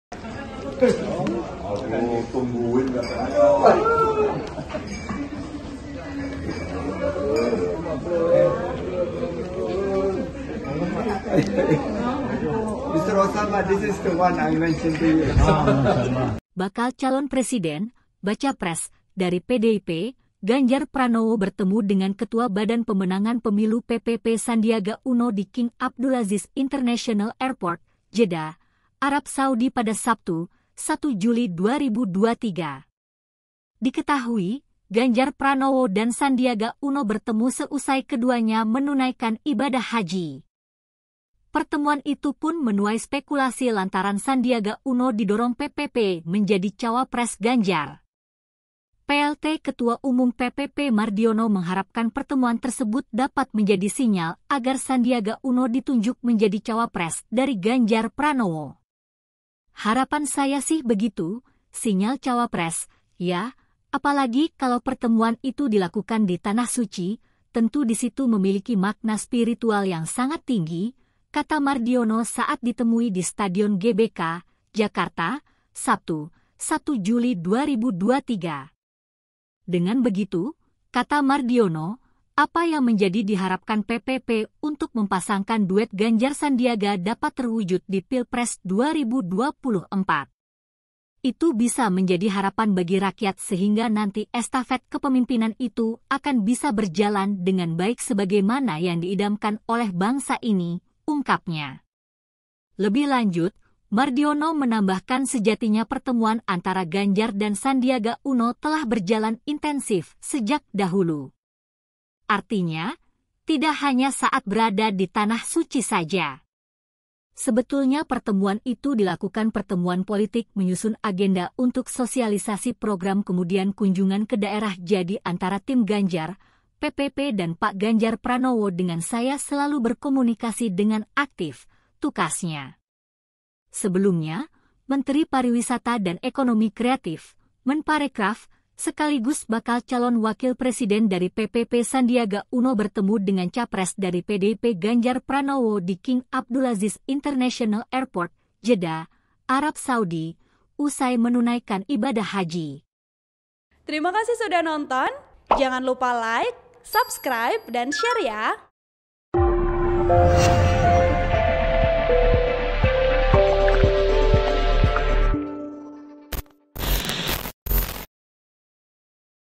Sandiaga Bertemu Capres Ganjar di Makkah, PPP Berharap Sinyal Jadi Cawapres
TRIBUN-VIDEO.COM - Bakal calon presiden (bacapres) dari PDIP, Ganjar Pranowo bertemu dengan Ketua Badan Pemenangan Pemilu PPP Sandiaga Uno di King Abdulaziz International Airport, Jeddah, Arab Saudi pada Sabtu (1/7/2023).

Diketahui, Ganjar Pranowo dan Sandiaga Uno bertemu seusai keduanya menunaikan ibadah haji. Pertemuan itu pun menuai spekulasi lantaran Sandiaga Uno didorong PPP menjadi cawapres Ganjar.

Plt Ketua Umum PPP Mardiono mengharapkan pertemuan tersebut dapat menjadi sinyal agar Sandiaga Uno ditunjuk menjadi cawapres dari Ganjar Pranowo.

"Harapan saya sih begitu (sinyal cawapres) ya, apalagi kalau pertemuan itu dilakukan di Tanah Suci, tentu disitu memiliki makna spiritual yang sangat tinggi," kata Mardiono saat ditemui di Stadion GBK, Jakarta, Sabtu (1/7/2023).

Dengan begitu, kata Mardiono, apa yang menjadi diharapkan PPP untuk mempasangkan duet Ganjar-Sandiaga dapat terwujud di Pilpres 2024.

"Itu bisa menjadi harapan bagi rakyat sehingga nanti estafet kepemimpinan itu akan bisa berjalan dengan baik sebagaimana yang diidamkan oleh bangsa ini," ungkapnya.

Lebih lanjut, Mardiono menambahkan sejatinya pertemuan antara Ganjar dan Sandiaga Uno telah berjalan intensif sejak dahulu. Artinya, tidak hanya saat berada di tanah suci saja.

"Sebetulnya pertemuan itu dilakukan pertemuan politik menyusun agenda untuk sosialisasi program kemudian kunjungan ke daerah jadi antara tim Ganjar, PPP dan pak Ganjar Pranowo dengan saya selalu berkomunikasi dengan aktif," tukasnya.

Sebelumnya, Menteri Pariwisata dan Ekonomi Kreatif (Menparekraf) sekaligus bakal calon wakil presiden dari PPP Sandiaga Uno bertemu dengan capres dari PDI-P Ganjar Pranowo di King Abdulaziz International Airport, Jeddah, Arab Saudi, usai menunaikan ibadah haji.

Dalam keterangan yang diterima, Sabtu (1/7/2023), Ganjar dan Sandi bertemu di ruang tunggu bandara pada pukul 02.37 waktu setempat. Terlihat Sandi mengenakan baju koko berwarna hijau, sementara Ganjar menggunakan kemeja putih.

Ganjar disebut sudah lebih dulu tiba di ruang tunggu untuk menanti keberangkatan pesawat yang membawanya kembali pulang ke Indonesia.

Rupanya, Sandi dan Ganjar menggunakan satu pesawat yang sama untuk kembali ke Tanah Air.

"Aku tungguin datangnya," ujar Ganjar. Ganjar dan Sandi langsung bersalaman ketika bertemu di ruang tunggu.

Selain itu, Ganjar dan Sandi turut memberi salam cipika-cipiki sembari tersenyum lebar.

Lalu, Sandi memperkenalkan salah satu pejabat di Arab Saudi kepada Ganjar. Dia lantas menyebut Ganjar sebagai Presiden selanjutnya.

"Mr Osama, this is the one i mention to you, Insya Allah the next President," kata Sandi.

"Dari Kementerian (Arab Saudi)," lanjutnya sambil memperkenalkan pejabat yang mengenakan jubah tradisional yang biasa disebut Bisht.

Istri Sandi, Nur Asia Uno, yang turut hadir dalam pertemuan tersebut turut bersalaman dengan Ganjar dan keluarga.

"You have to visit (Indonesia), it is so beautiful," kata Sandi.

"Insya Allah," jawab pejabat di Kementerian Arab Saudi tersebut.

Selanjutnya, Sandi, Ganjar, dan para pejabat di Kementerian Arab Saudi tampak berbincang sembari berdiri menunggu dipanggil untuk keberangkatan pesawat.

Sebelum take off menuju ke pesawat, Sandi sempat berfoto dengan petugas Bandara King Abdulaziz didampingi pejabat Kementerian Kerajaan Arab Saudi.